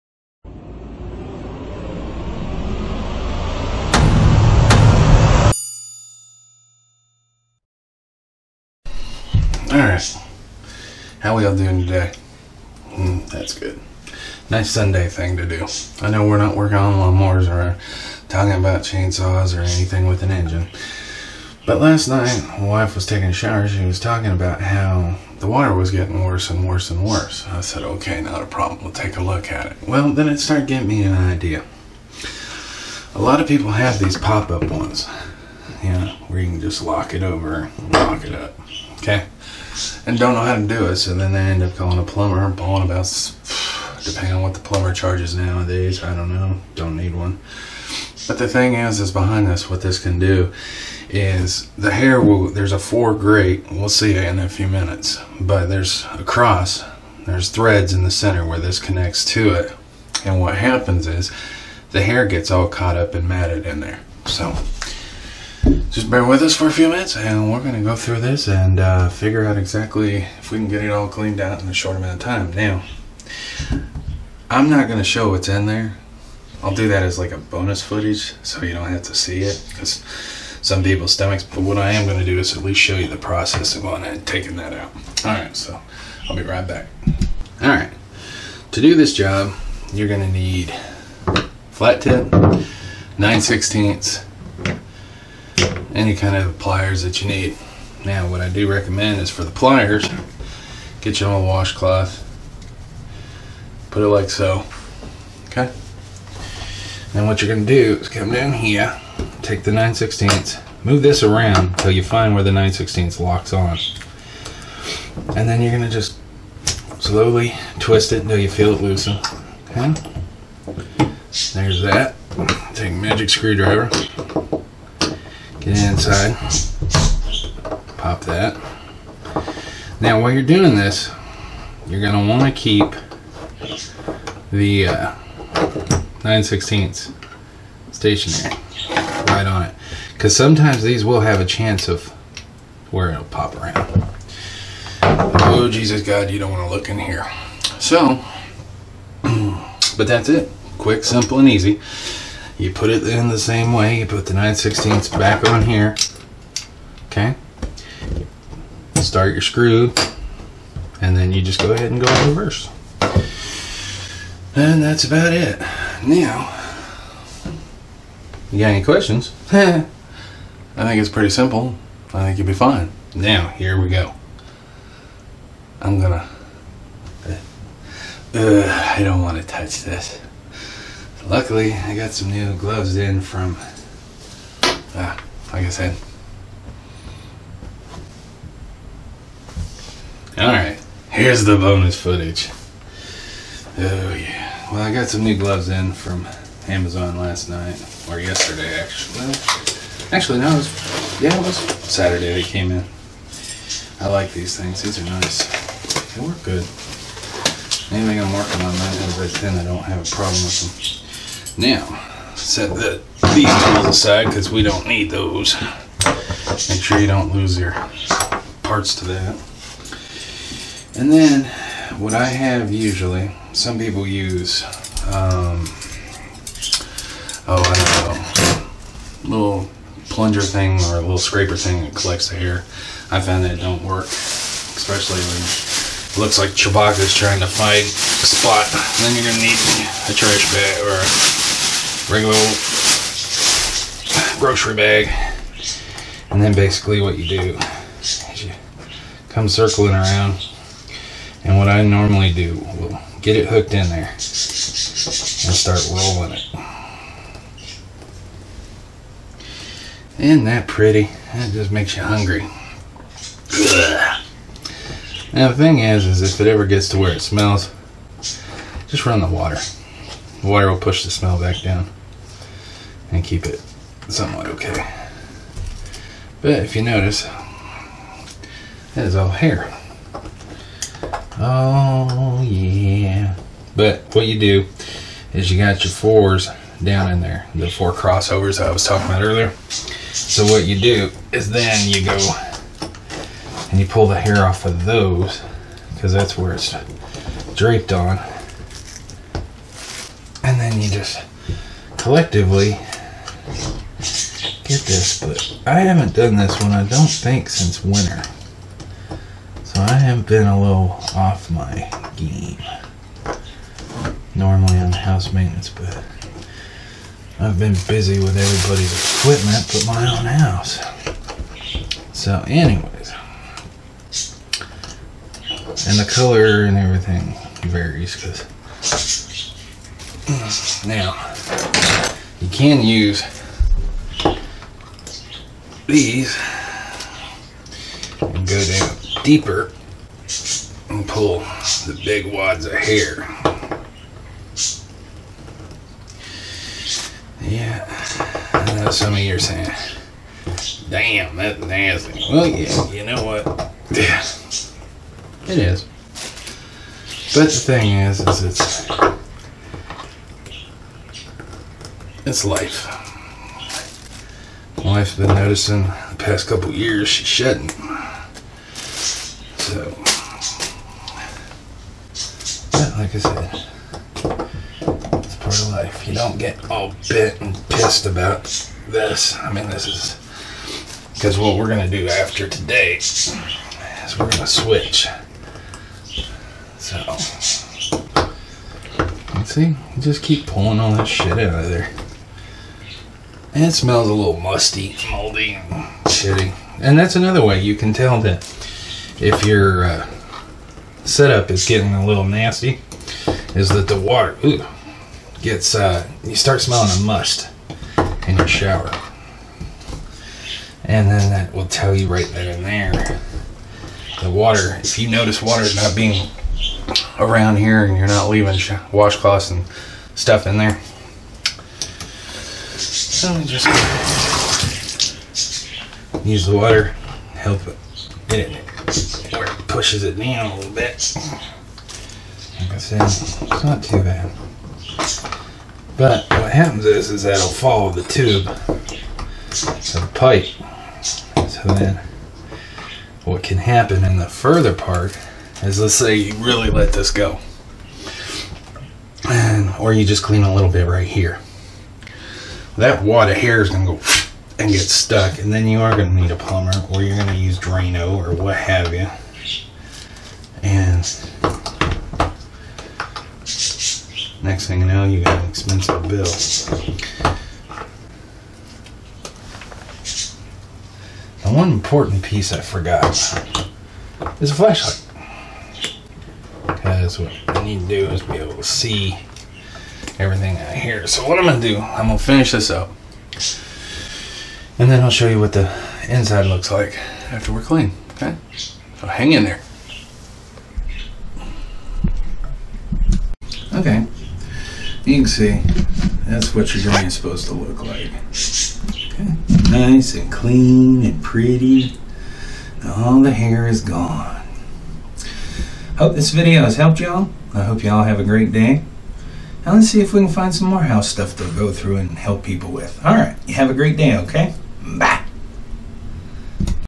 Access to a hair clog in the tub drain. by remove and reinstall tub drain stopper.
How to remove the pop lock plug on a bathtub. Bonus at the end is the hair removed. After the removal and reinstall, you'll see all the hair that was is in the drain :)